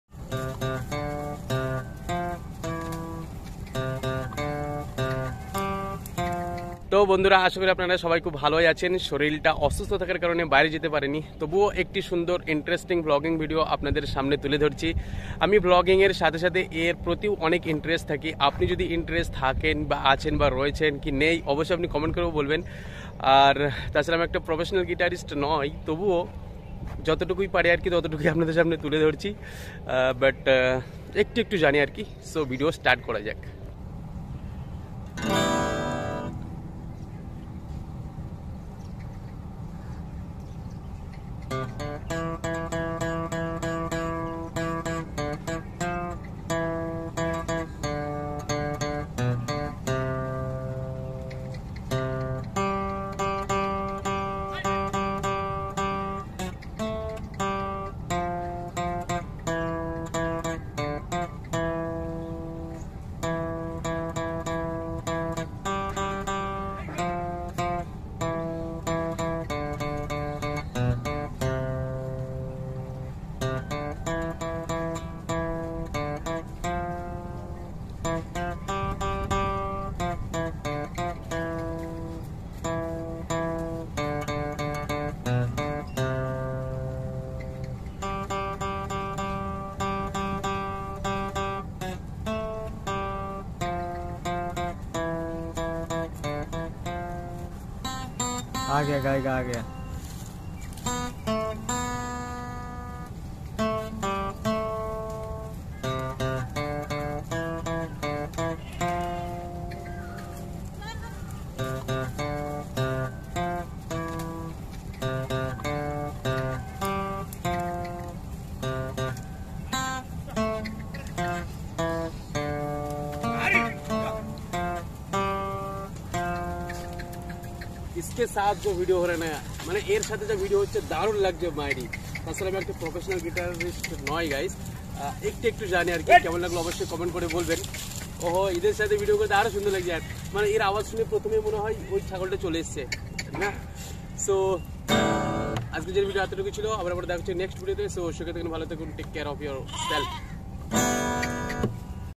Guitar 🎸 play with animal's 🐂 পশুদের সাথে গিটার বাজালাম || Rabiul Bhai
today in this video i show you Guitar 🎸 play with animal's i hope you enjoyed this video